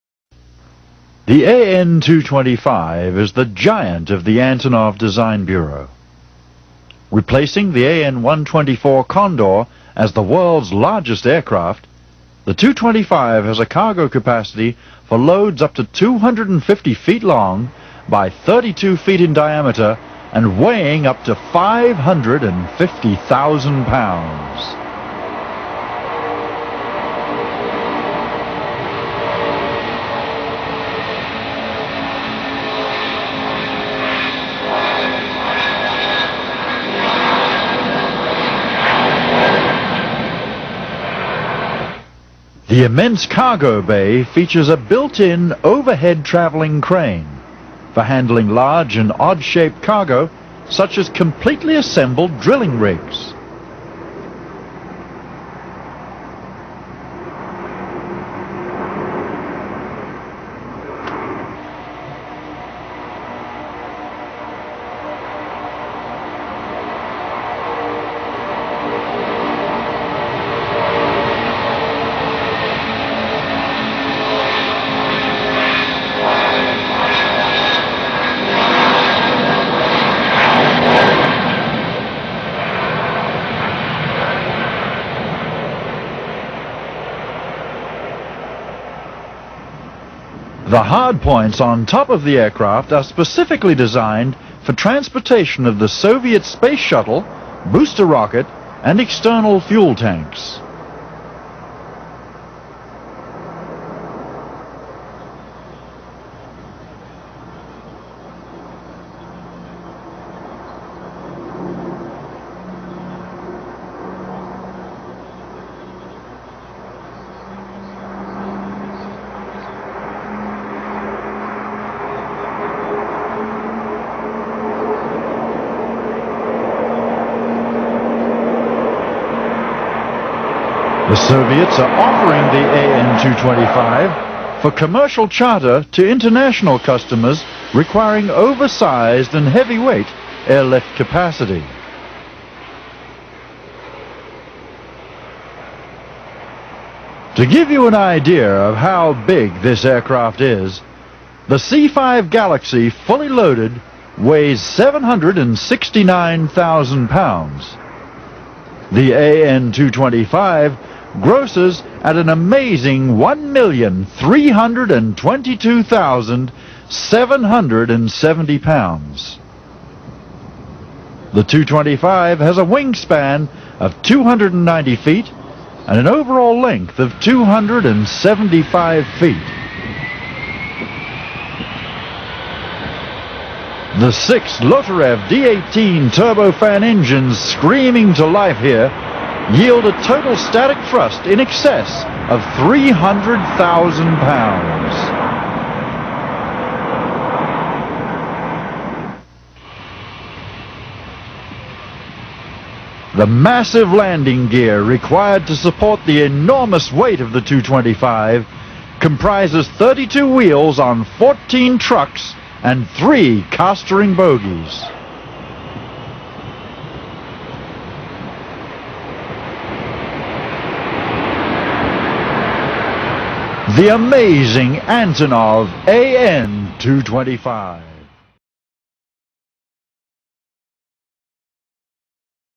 A225 The Biggest and Largest Aircraft in the World by BlueSky Crew
The Antonov 225 is currently the largest flying aircraft in terms of wingspan and length, as well as overall cabin volume. The An-225 is 84m long, with a wingspan of 88.4m and a payload of 250,000kg.